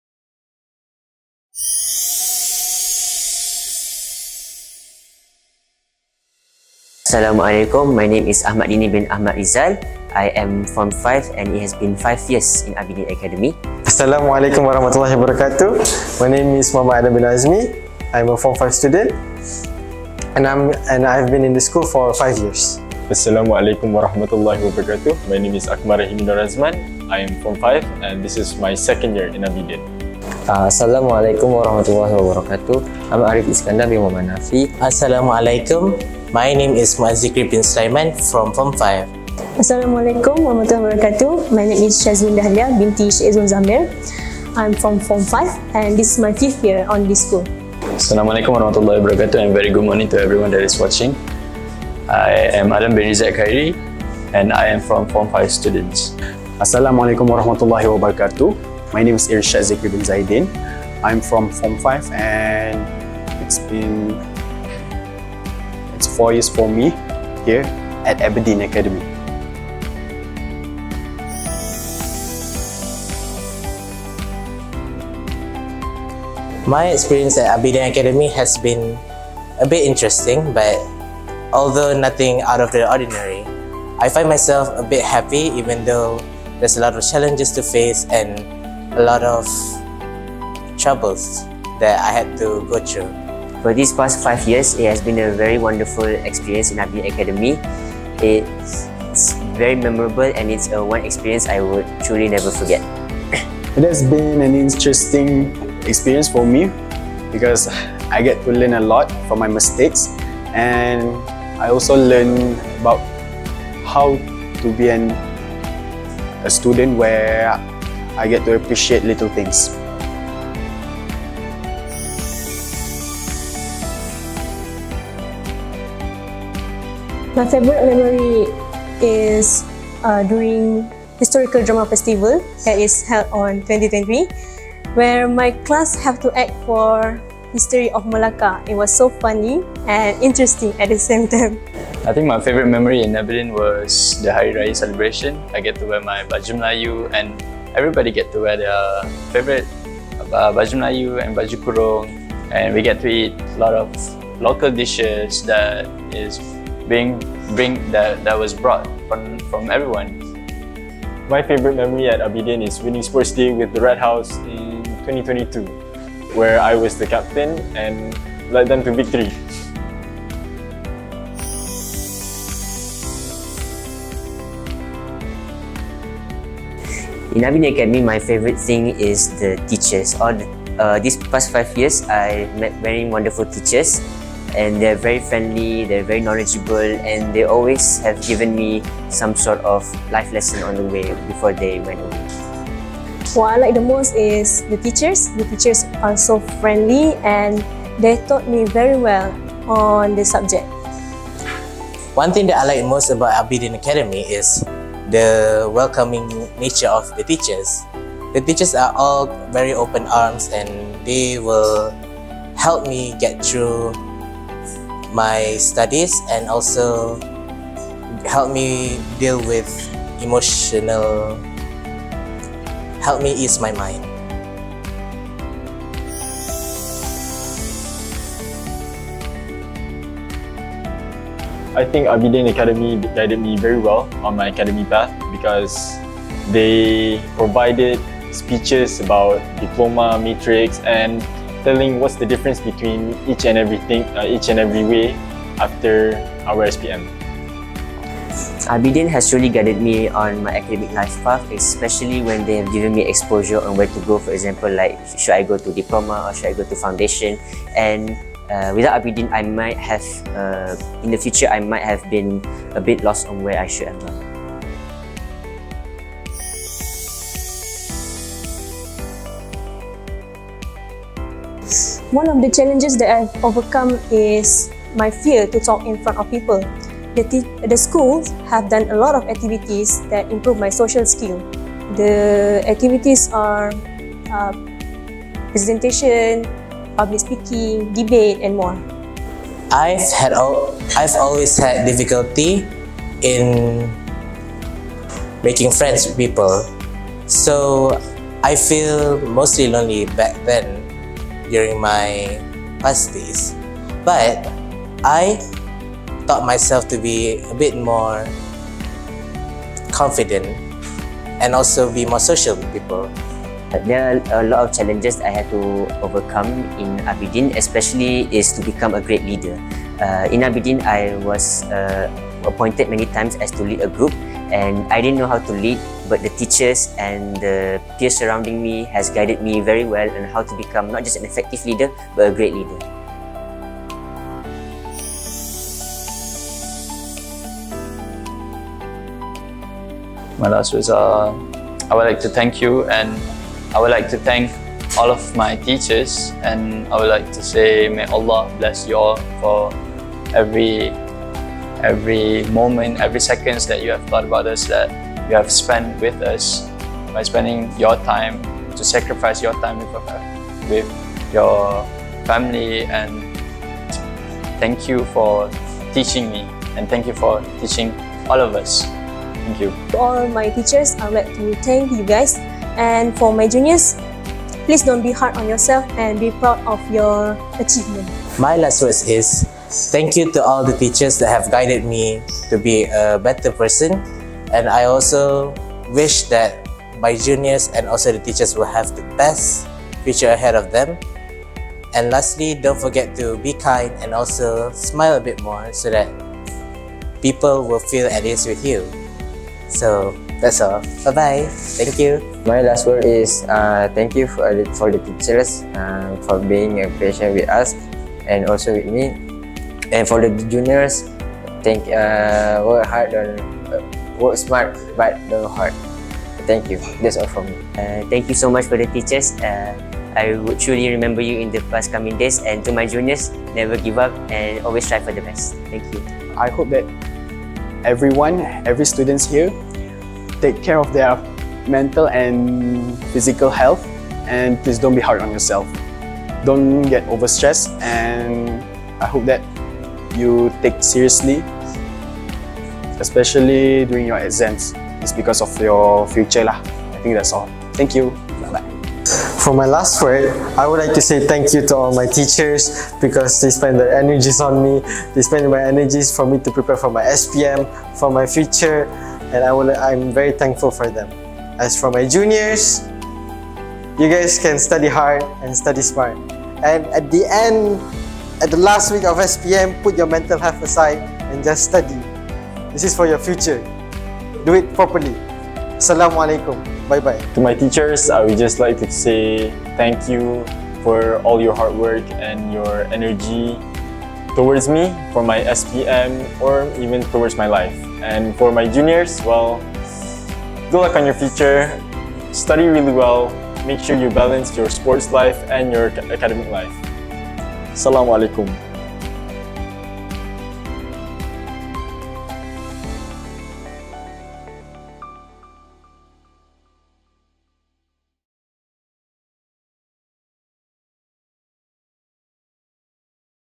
Abedeen Academy Private School 2023 Ihtifal Testimony | Secondary Students
Let's hear what our secondary school students say before they embark on a new journey.